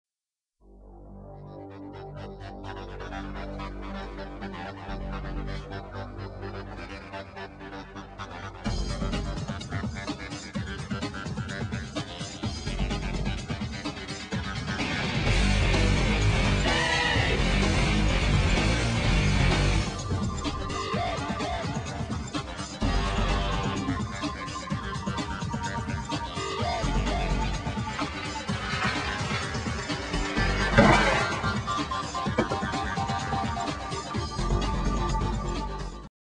The Box Trick
Optical illusion of a girl unwittingly climbing into a box about to be hit by car.